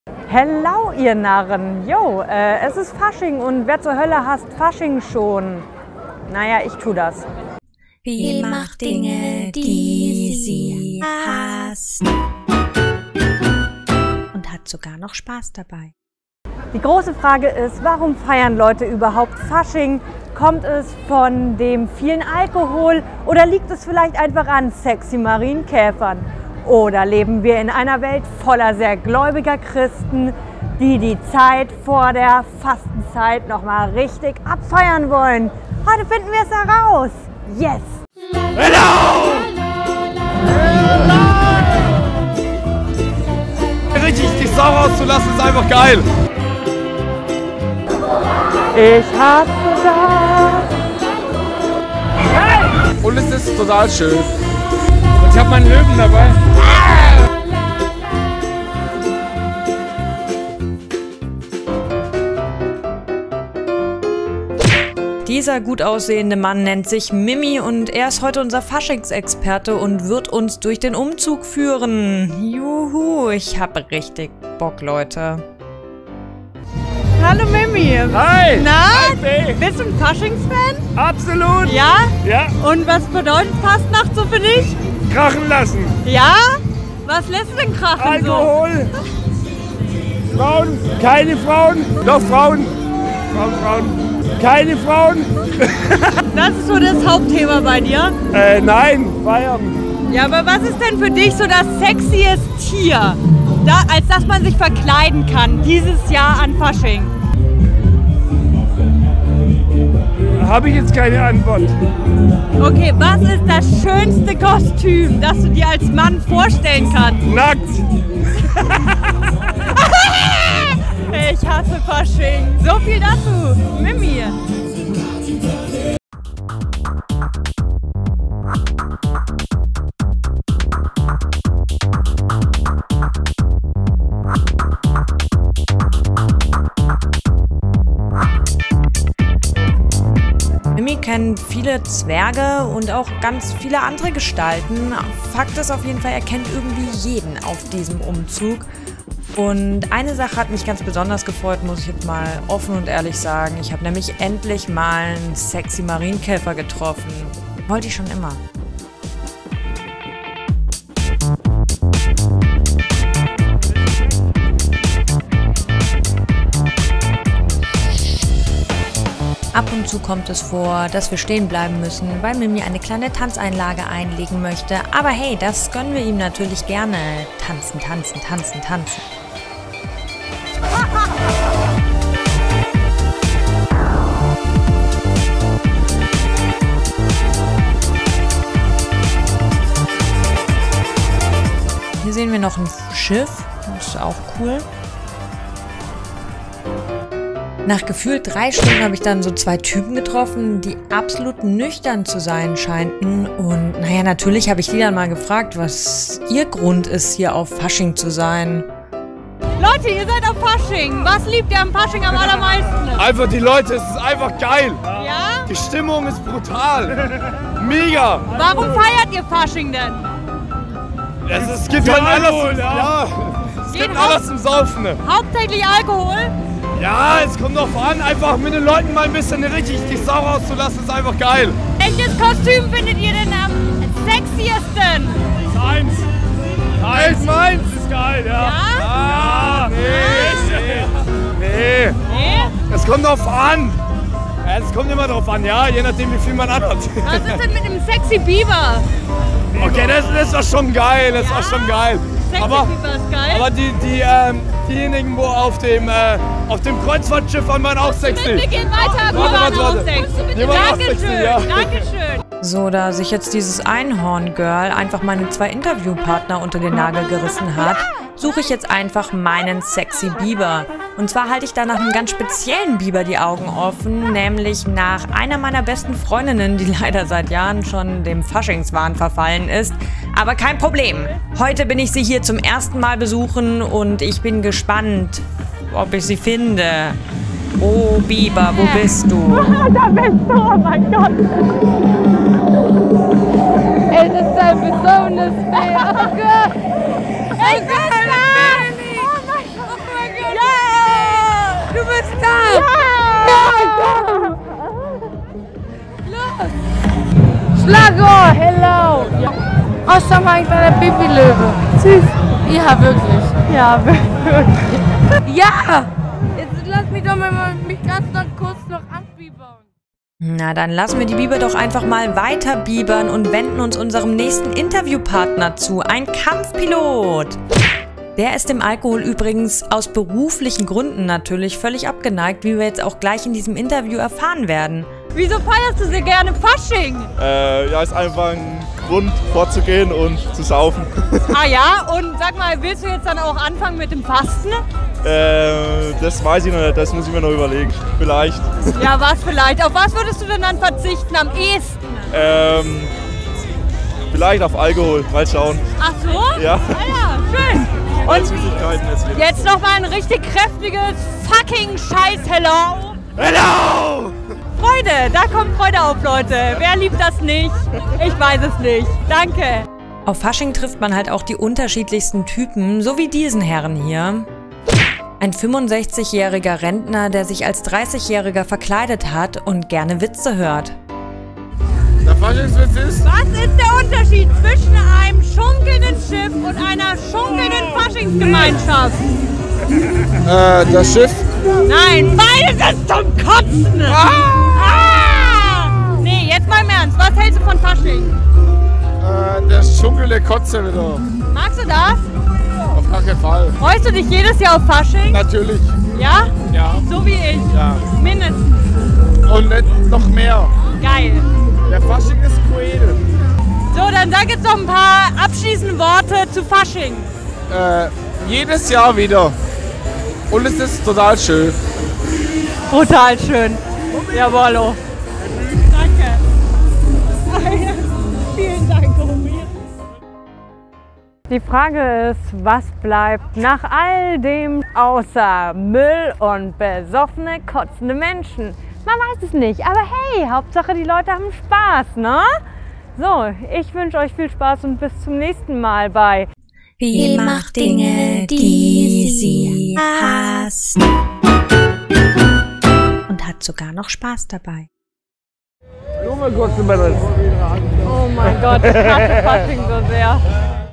Fee macht Dinge, die sie hasst (Fasching)
➤ Helau Alaaf, ai Narrenhass.
Es war hart aber ich habe es gewagt: ich war auf einem Rosenmontagsumzug. Bei angenehmen 4°C und Nieselregen habe ich mit Faschingsfans gesprochen und versucht zu verstehen, was man an Fasching nicht hassen kann.

Das ist die erste Folge von "Fee macht Dinge, die sie hasst". Wenn ihr das mögt, mache ich vielleicht öfters Dinge, die ich hasse. Also: lasst es mich wissen!